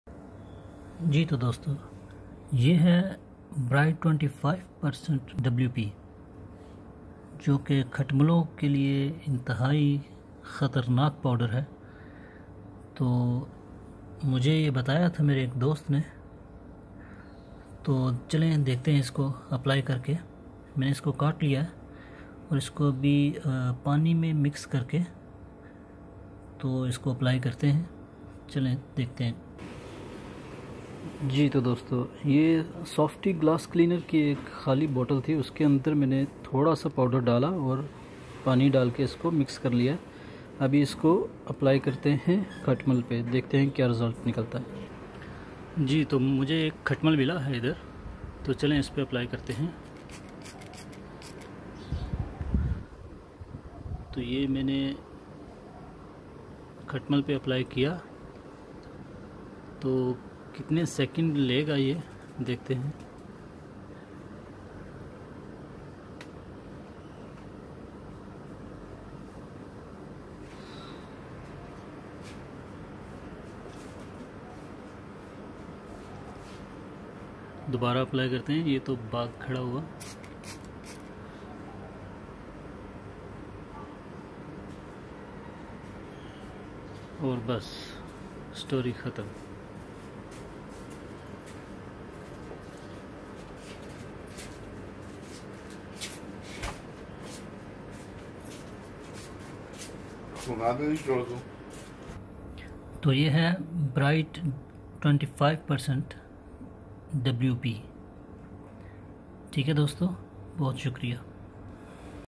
Bright 25% WP | Bedbugs Killer | Khatmal Mar Powder | کھٹمل مارنے والا پاؤڈر
Get rid of bedbugs. Apply Bright 25% WP to the infected area and feel free of bedbugs.
Use only powder or use it with mixing in water. It is effective in both the cases.